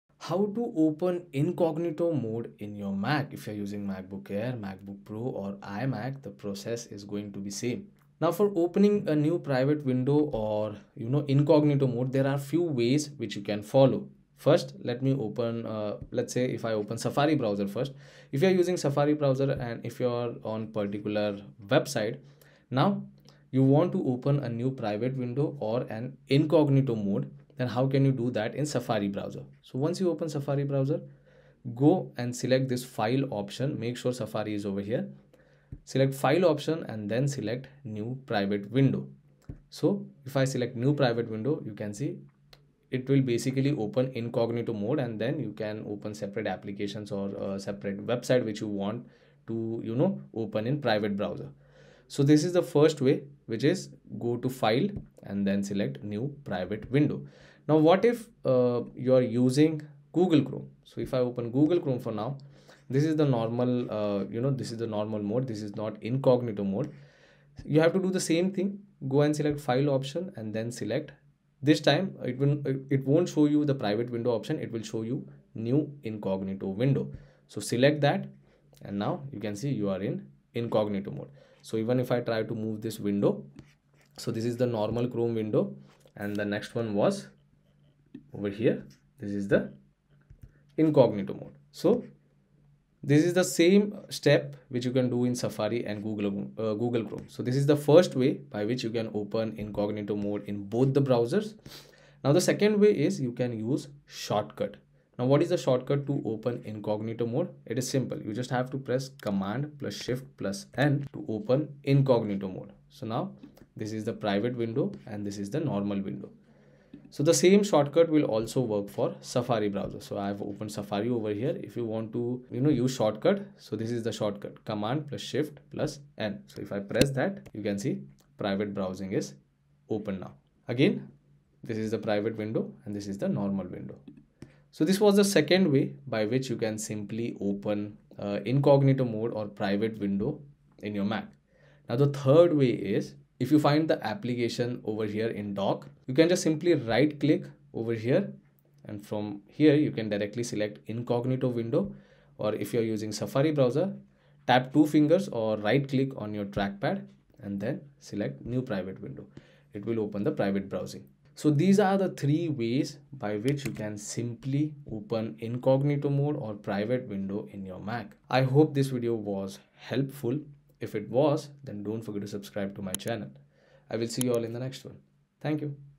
How to Open Incognito Mode in MacBook - Mac Safari Browser Incognito
Here is how to go to incognito mode in a Mac. The incognito mode or private mode does not store history and hence considered more private than the normal browsing in Safari, Brave, Chrome, Firefox or other browsers on Mac. Let's learn the shortcuts and the settings that allow you to enter such a private mode on any MacBook or macOS running device.